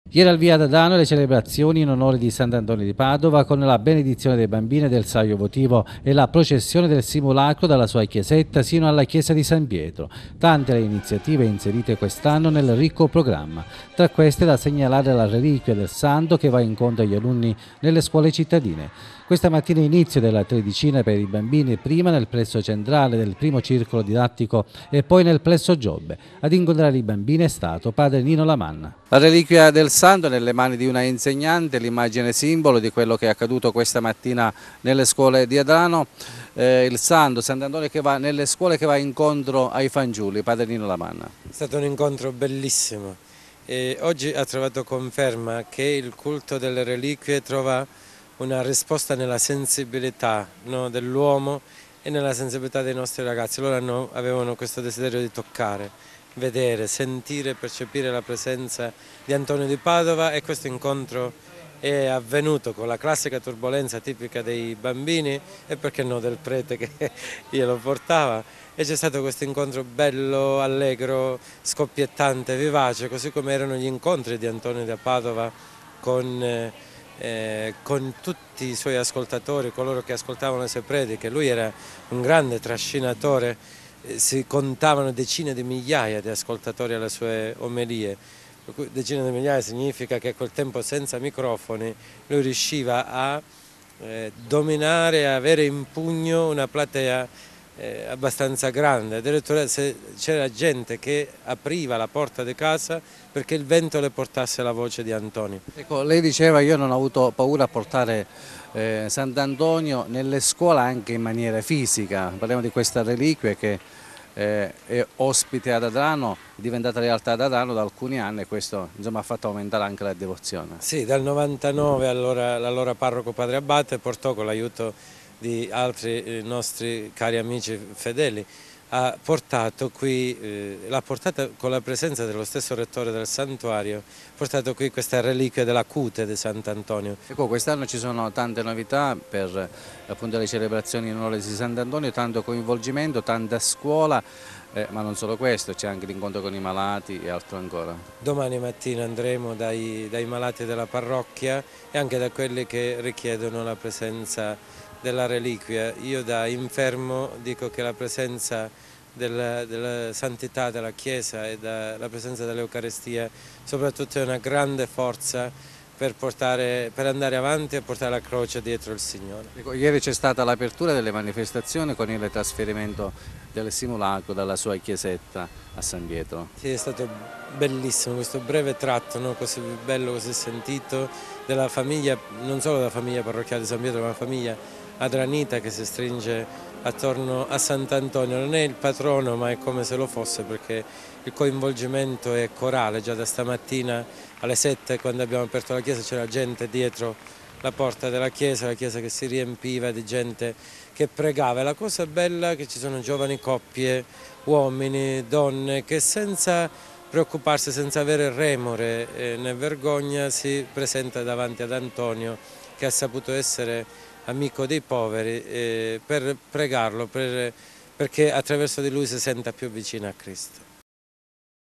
La reliquia di Sant'Antonio di Padova va incontro agli alunni delle scuole
Al via ad Adrano le celebrazioni in onore di Sant'Antonio di Padova.
La reliquia del santo va incontro agli alunni nelle scuole cittadine.